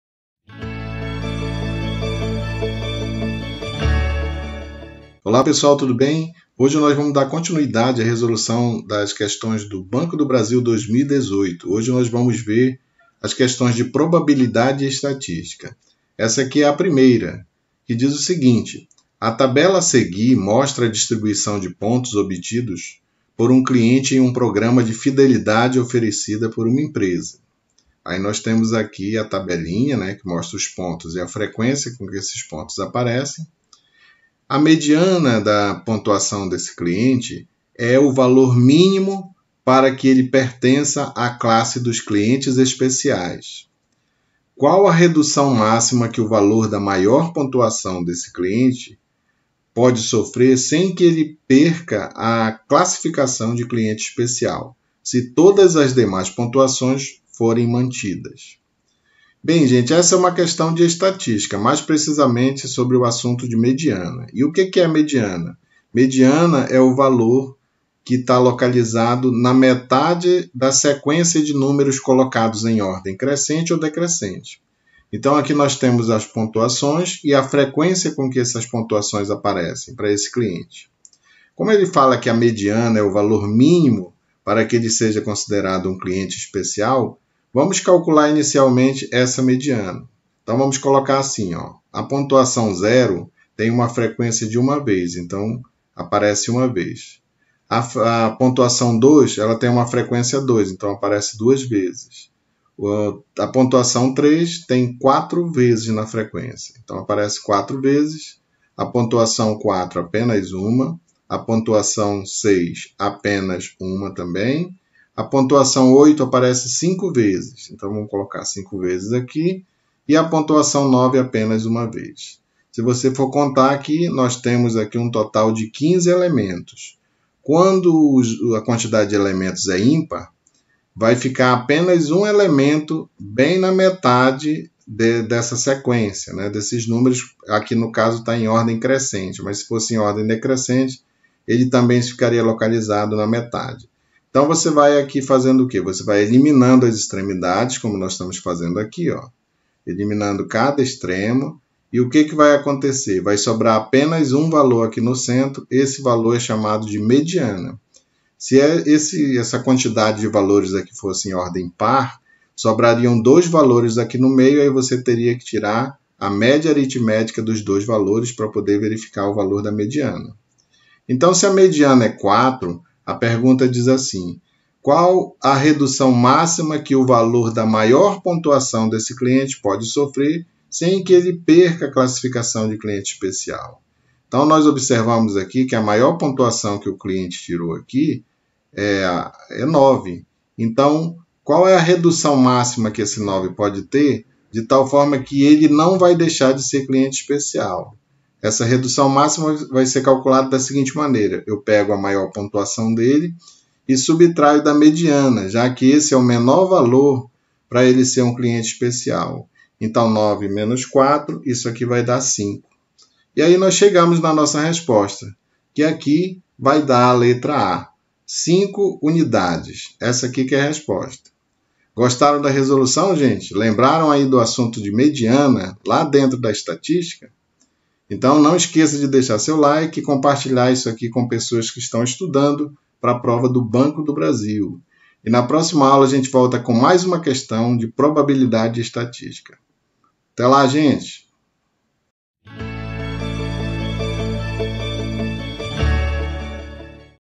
CONCURSO BANCO DO BRASIL 2021_Questão 01_Probabilidade_Estatística_Prova 2018
Olá pessoal, tudo bem? Vamos resolver a questão 01 de Probabilidade e Estatística do Concurso do Banco do Brasil 2018, para escriturário, desenvolvido pela CESGRANRIO. Se vc está se preparando para a prova de 2021, assista a resolução desta questão e observe o nível das questões de matemática.

Para assistir os vídeos anteriores da Prova de Matemática do Concurso Banco do Brasil 2018, clique nos links abaixo: